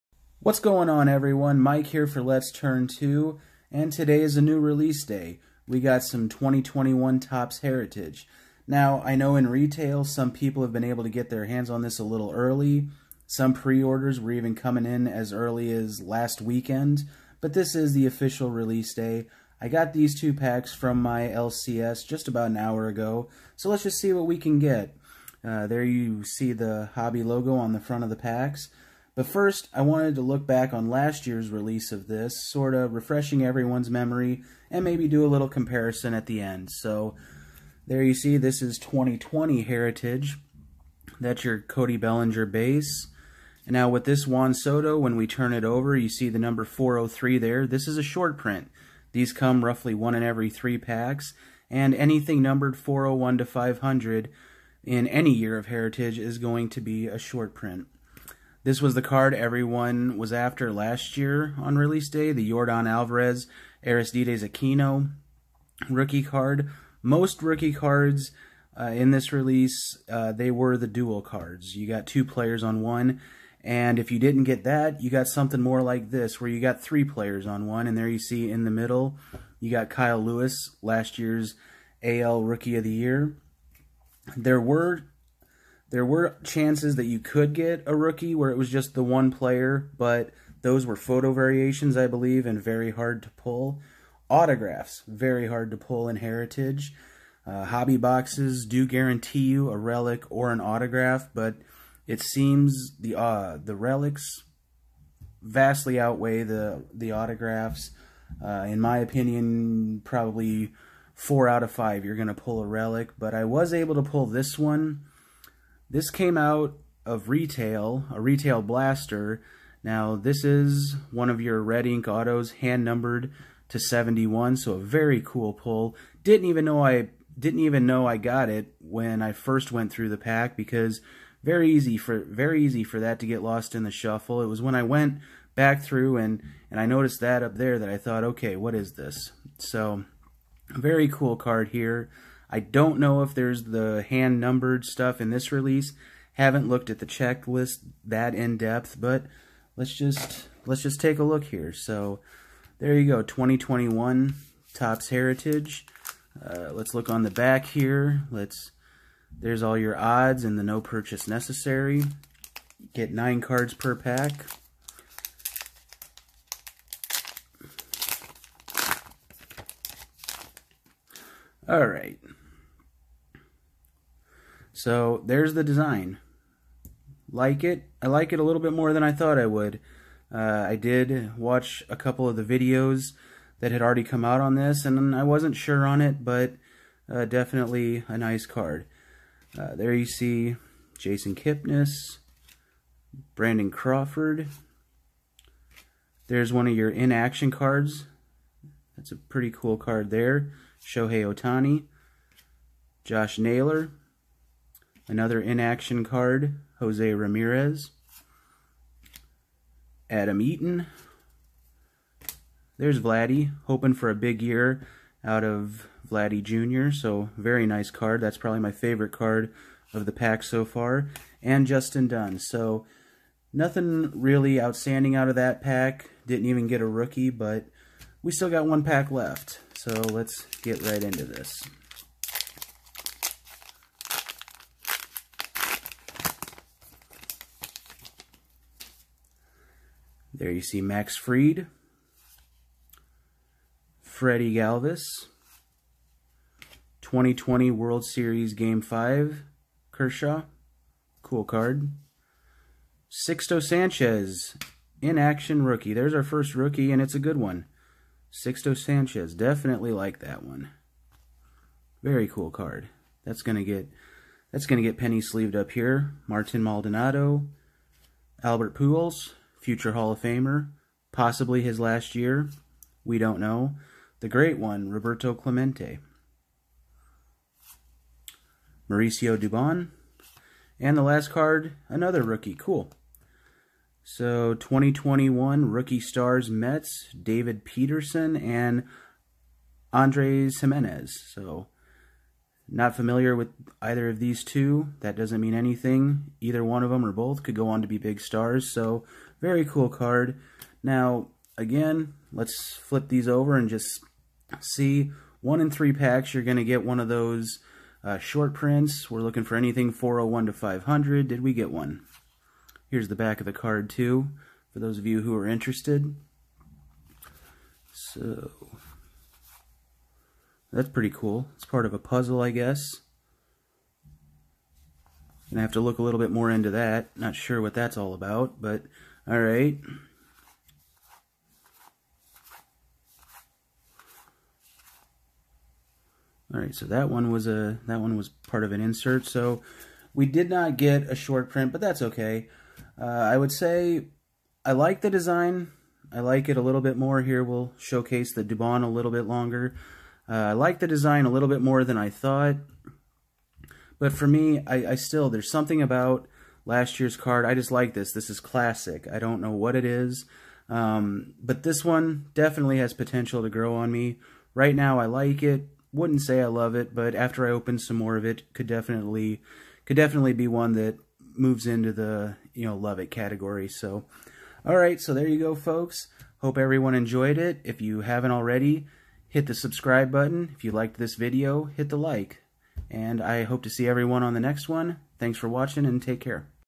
2021 Topps Heritage - New Release!
It's release day for 2021 Topps Heritage, and I'll be opening 2 packs from a hobby box. I'll then compare it to last year's release of the same product, give a review, and say which of the 2 I'd prefer if both were available for the same price.